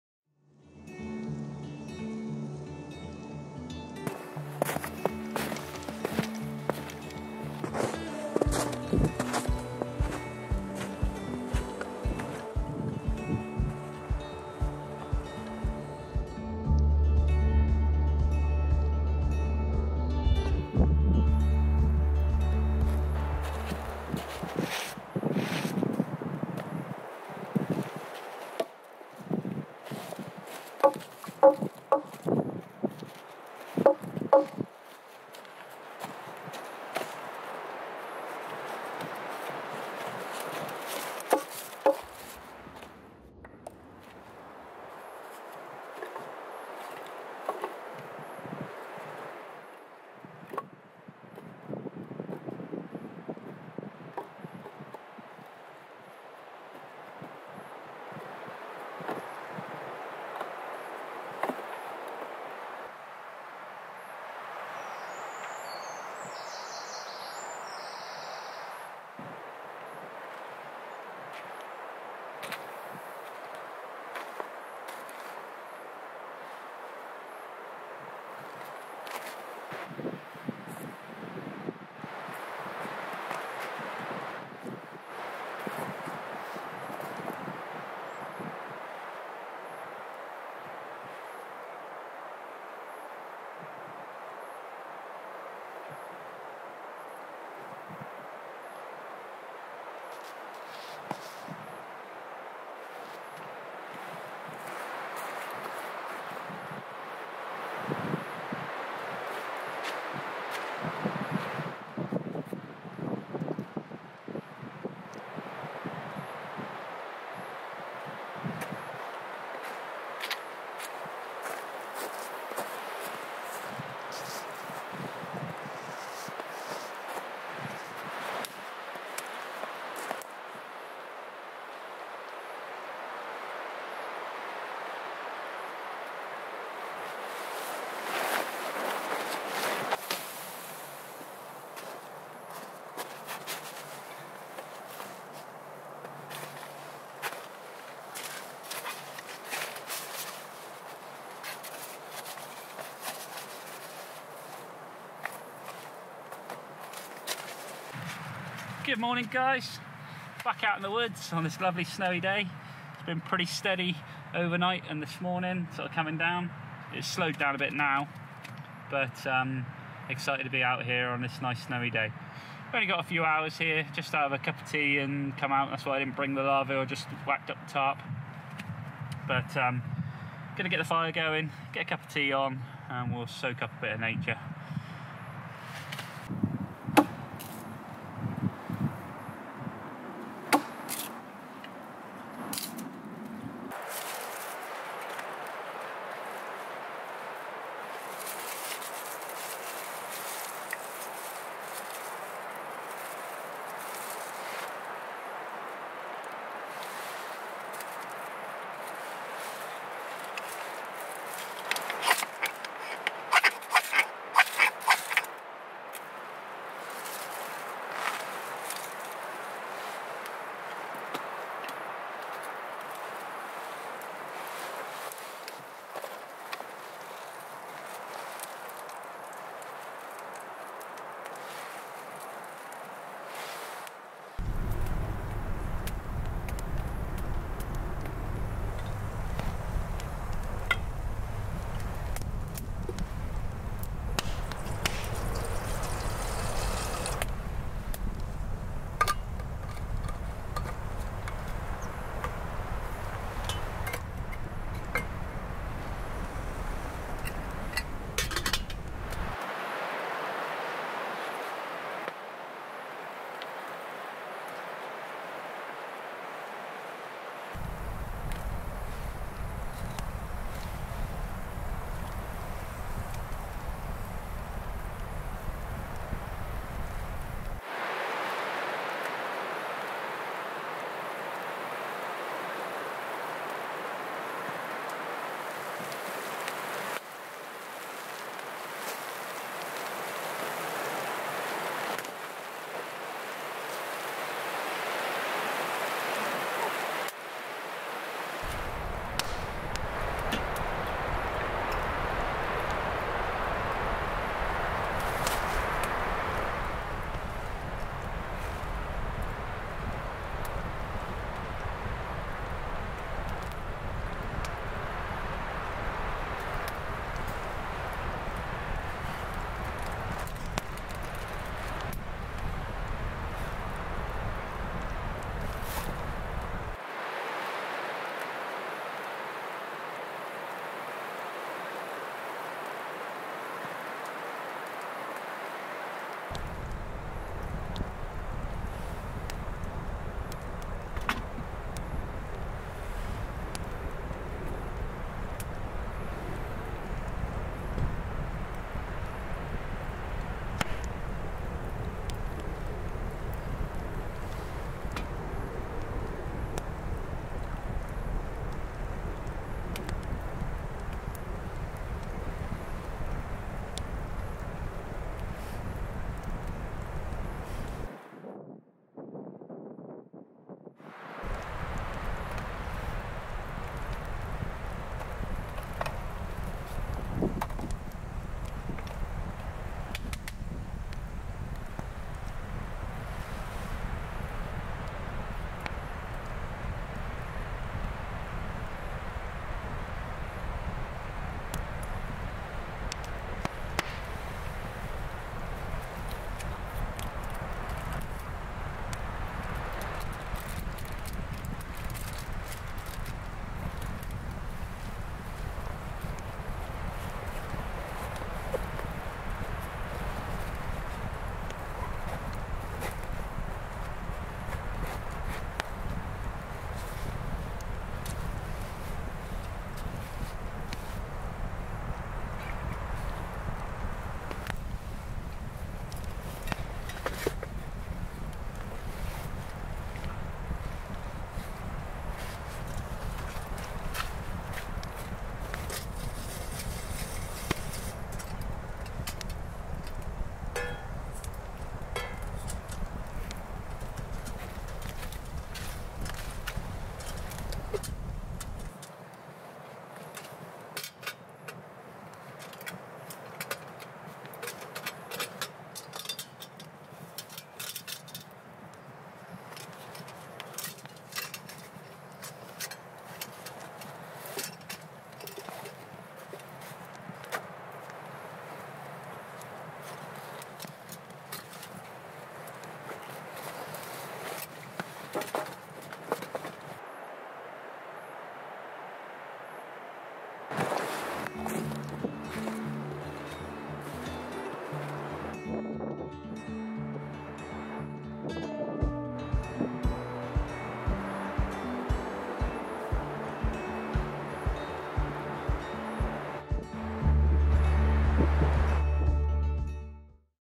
Snow falls as I soak up nature on a cold winters morning at the Bushcraft camp. tarp, Campfire,
A short trip out to the woodland, to have a tea, at the camp, and soak up nature, on a cold snowy, day.

The camp looks great covered in snow when I arrive. I take my time and set up Tarp. I enjoy listening to the soundtrack of nature as a snow storm hits the camp under my tarp, next to the campfire,

Alone in the woods on a day like this really clears your mind and makes you feel free.
I loved watching the trees sway in the wind and birds singing while warm next to the fire cooking a cup of tea.

You can follow the channel on Instagram here and get updates about the upcoming videos

T-H Bushcraft,